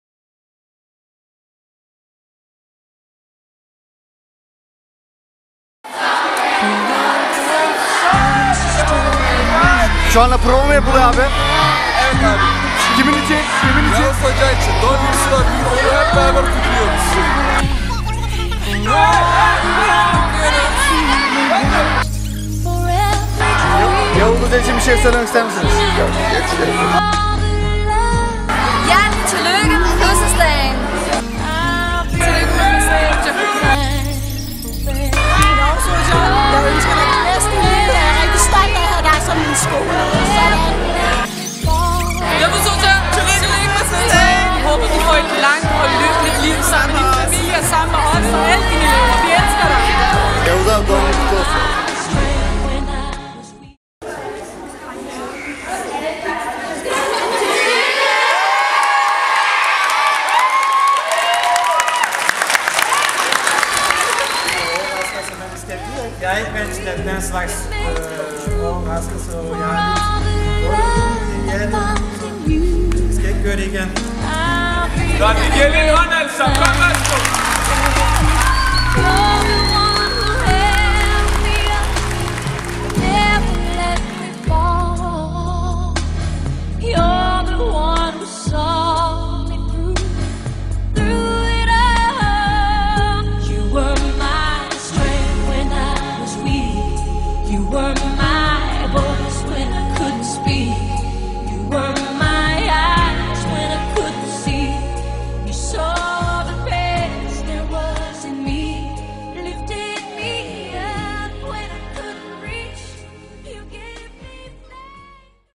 Tillykke med fødselsdagen
Htps fejrer skolelederens fødselsdag .
Lavet af T.O (TYROZK)